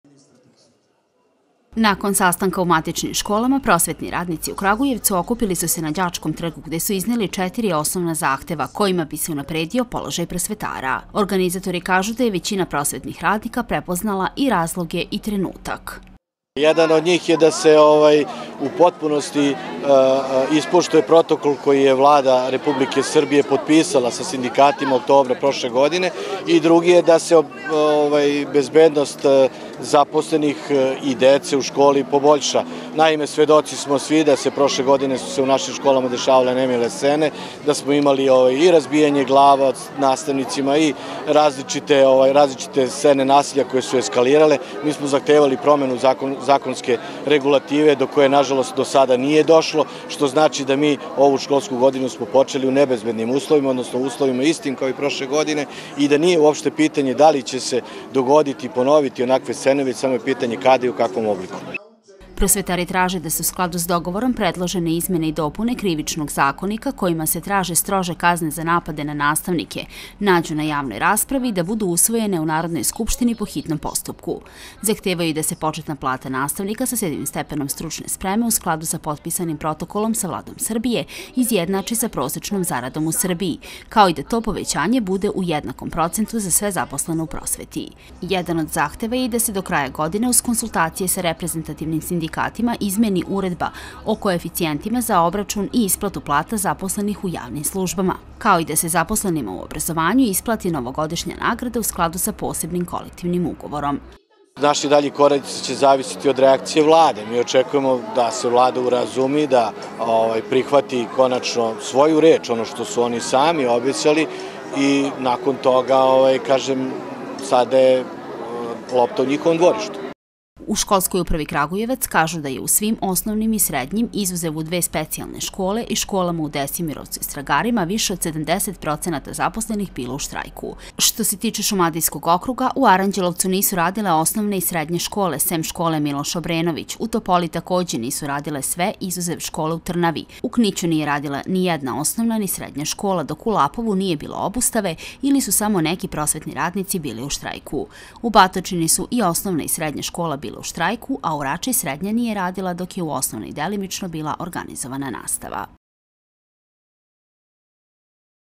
U Kragujevcu u štrajku 28 škola
U gotovo svim kragujevačkim školama danas nije bilo nastave.